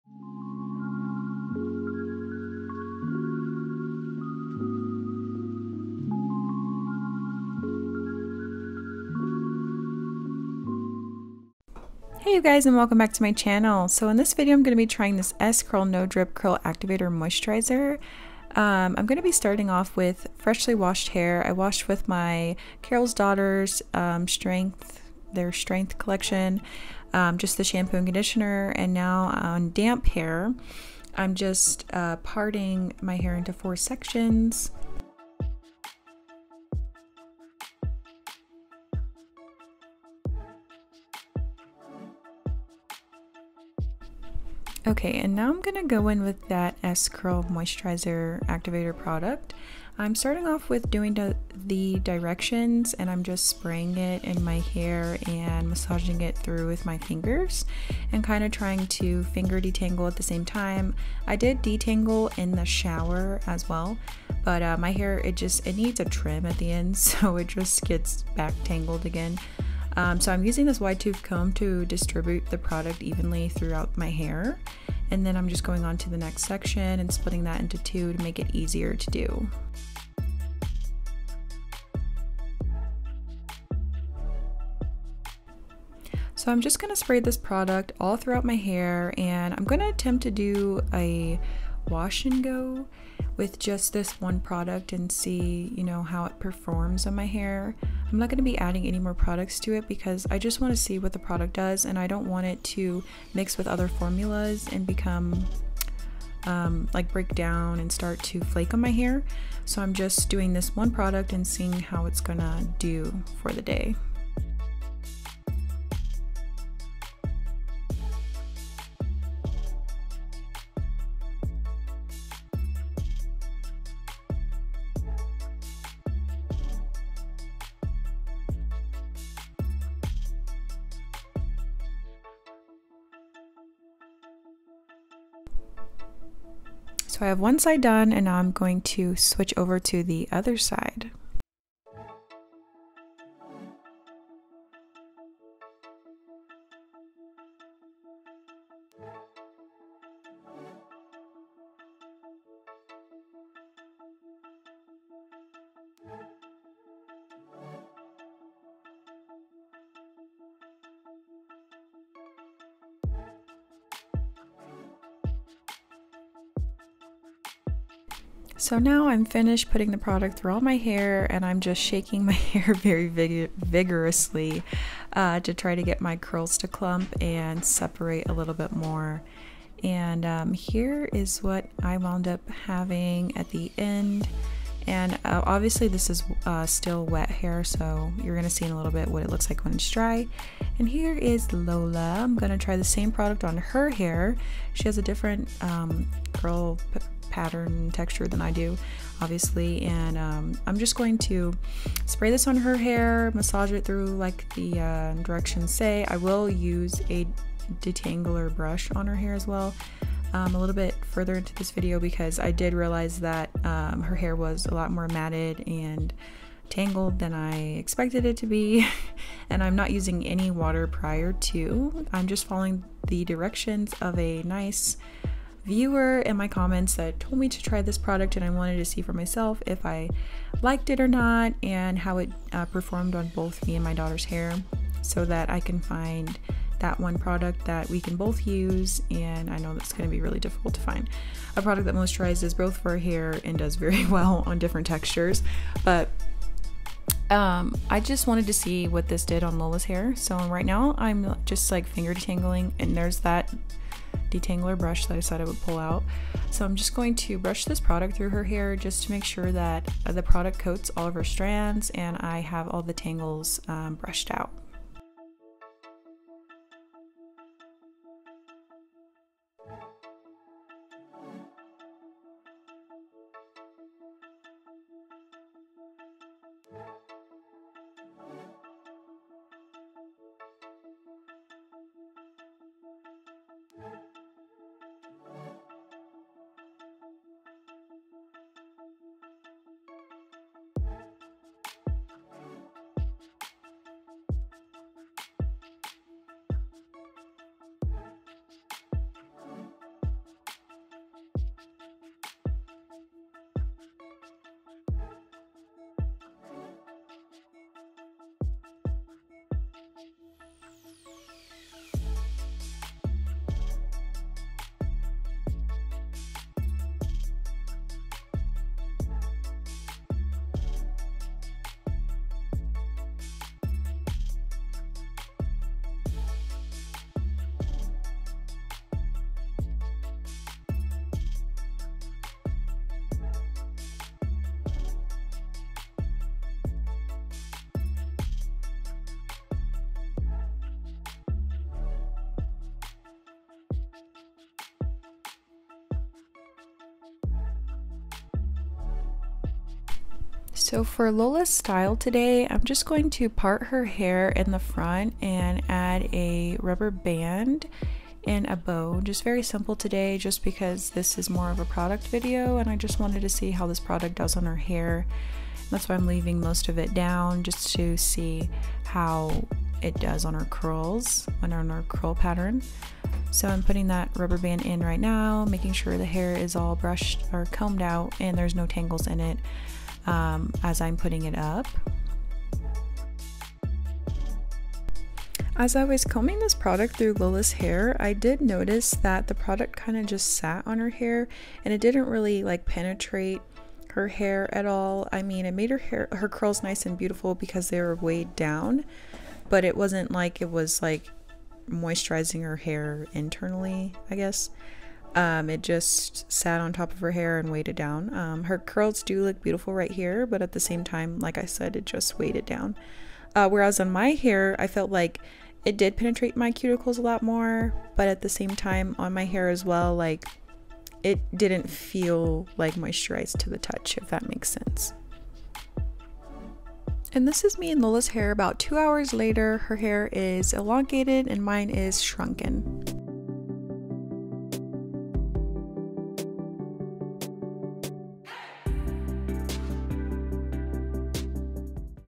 Hair Product Review | S Curl Activator Moisturizer | Same Product on 2 Different Textures
Hey Guys…In this video, I tried a product recommendation from one of my viewers. If you guys want to see me try any other products on mine and/or Lola’s hair let me know down in the comments. Thank you guys for all of your kind comments, love and support.